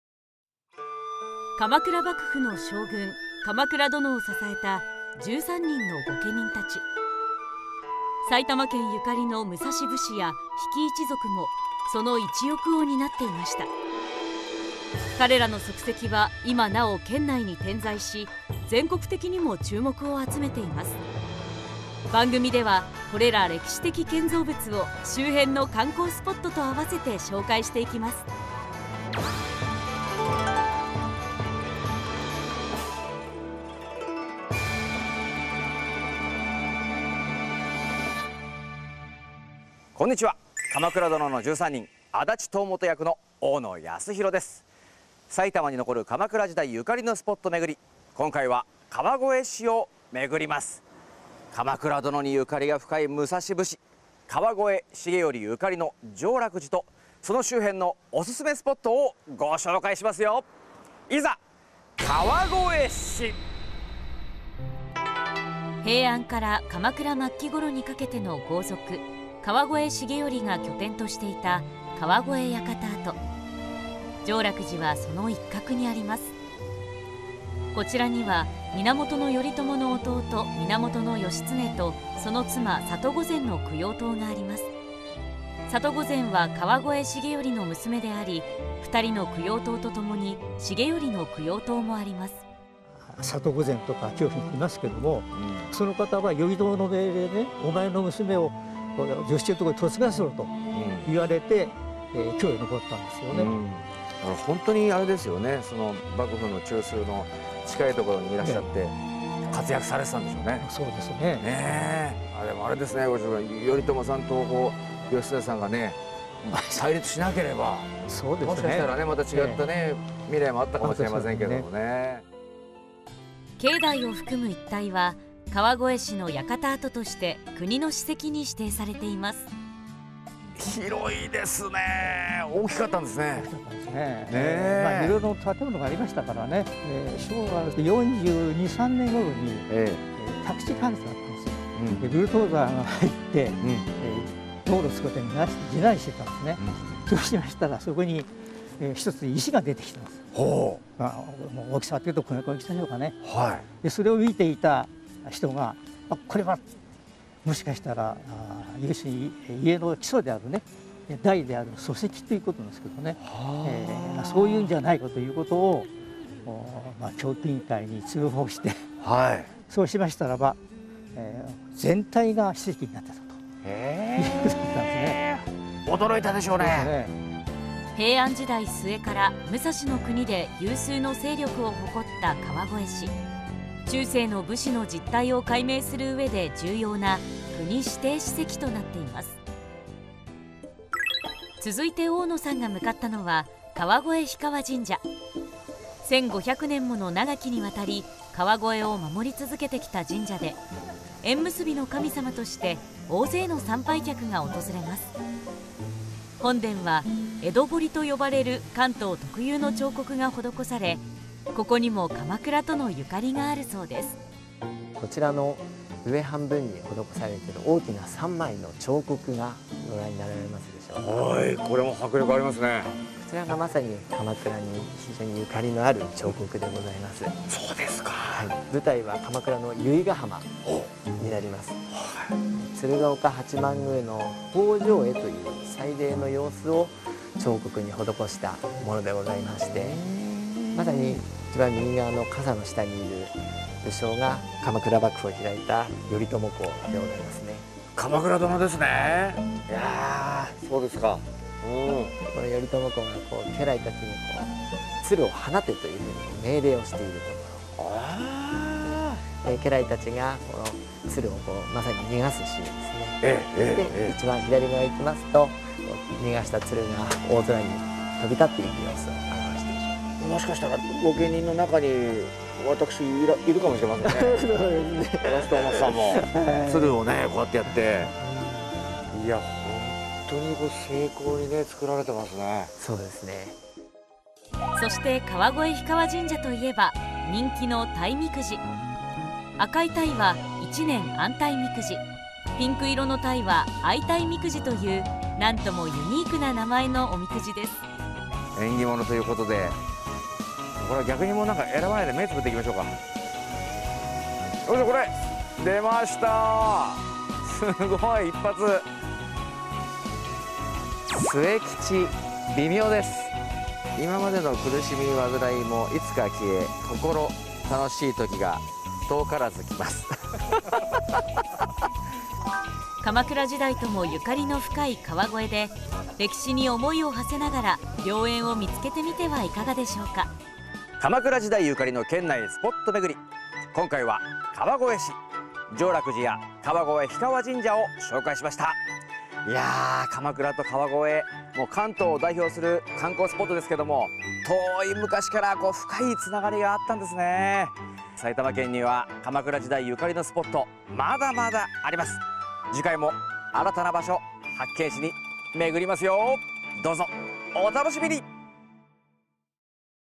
彩の国鎌倉めぐり（第９回 川越市）
大河ドラマで話題の鎌倉殿や彼等を支えた武蔵武士ゆかりの地が多く残されている埼玉県。
県内の鎌倉時代ゆかりの地と周辺のおすすめスポットを大河ドラマ出演俳優が巡ります。
第９回は川越市。比企一族や鎌倉殿ゆかりのスポットを紹介します！
歴史に思いを馳せながら、彩の国の鎌倉を訪れてみませんか？

♪2022年10月1日～2023年1月15日までデジタルスタンプラリーを開催中♪
▼「彩の国　鎌倉めぐり」について
▼埼玉県公式観光サイト「ちょこたび埼玉」